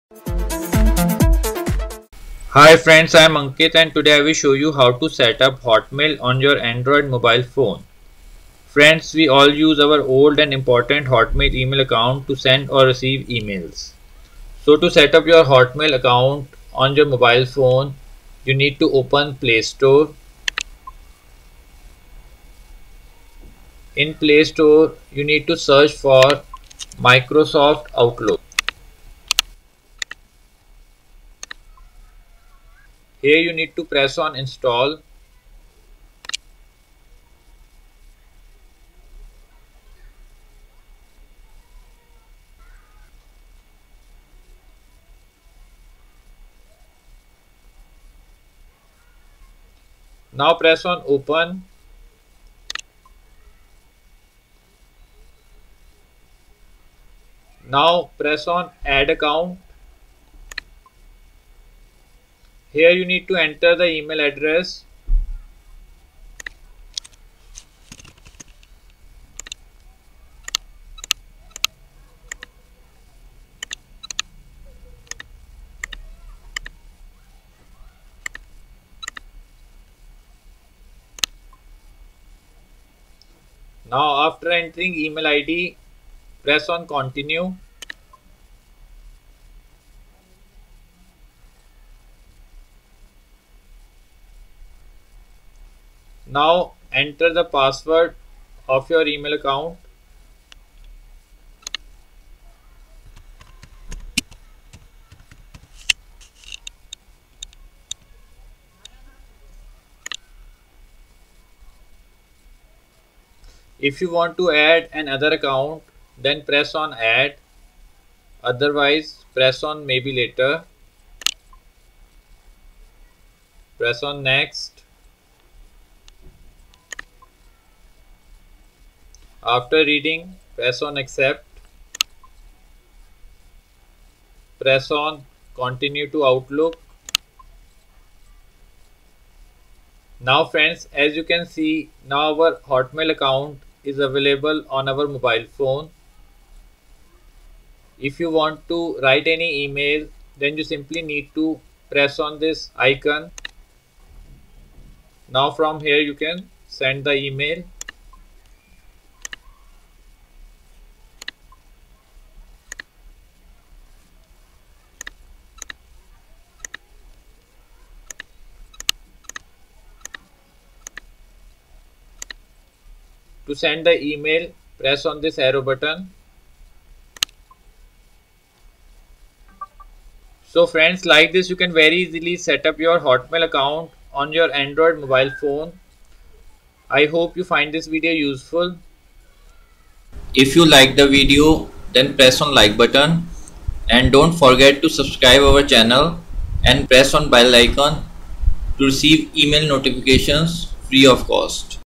How to Set Up Hotmail on Android
In this video we have shown How to Set Up Hotmail on Android Step by Step